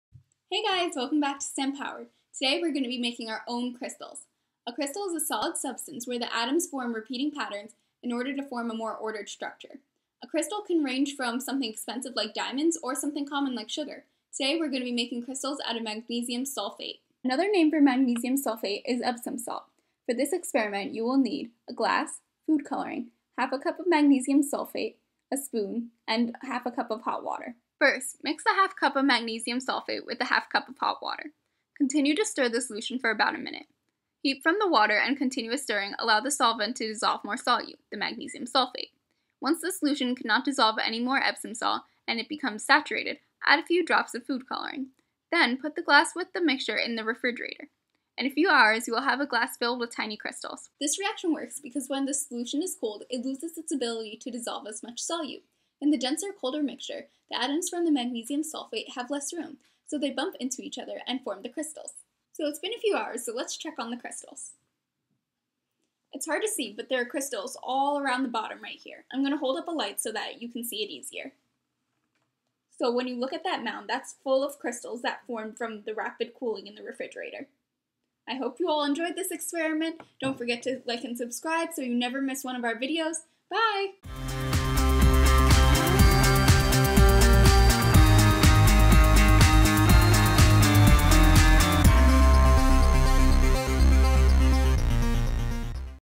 How to make magnesium sulfate crystals!
With just a few household ingredients you can make beautiful crystals in just a few hours.